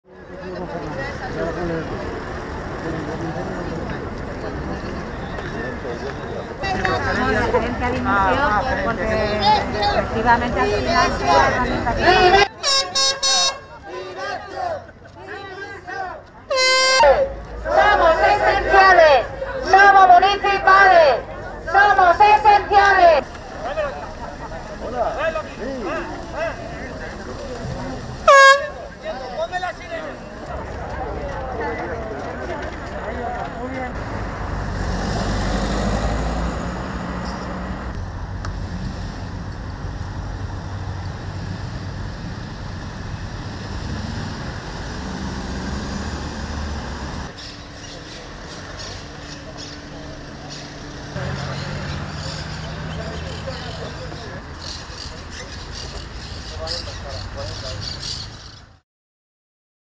Virus: les employés des cimetières de Madrid en grève | AFP
Le personnel du funérarium municipal de Madrid fait grève et manifeste le jour de la Toussaint aux portes du cimetière de Carabanchel au sud de la capitale, pour demander plus de personnel et de ressources afin de faire face à l'augmentation du nombre de décès provoqués par la deuxième vague de la pandémie de coronavirus.